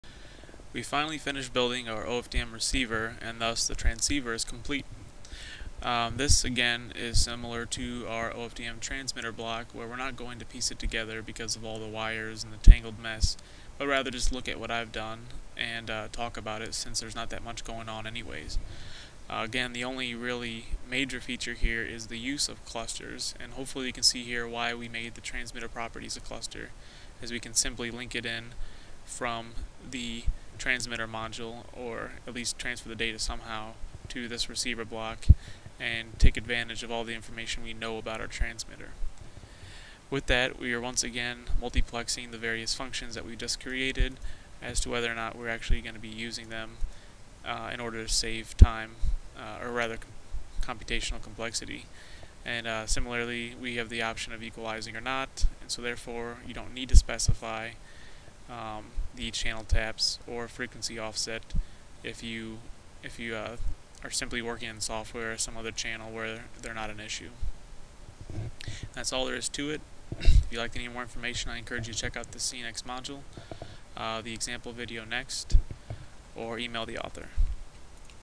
OFDM Symbol Decoder LabVIEW Tutorial
This is a LabVIEW tutorial on implementing the OFDM symbol decoder sub-VI for an OFDM receiver. The full project can be found at: